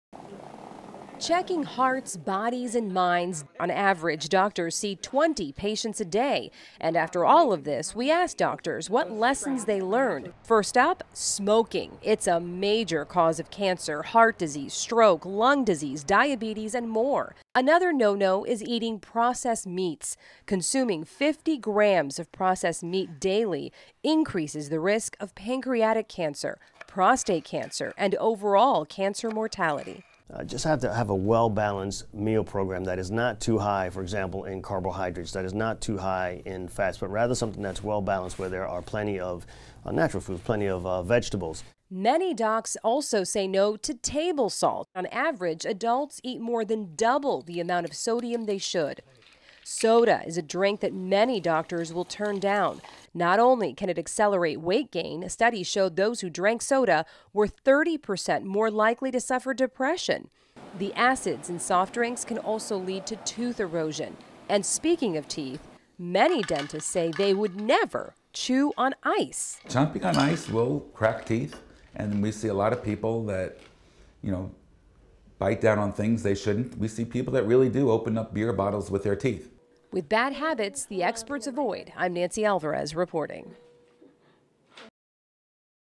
LIFE-SAVING LESSONS LEARNED: WHAT YOUR DOCTOR WON’T DO!
What bad habits are your doctors avoiding? Here’s how you can adapt them into a healthier lifestyle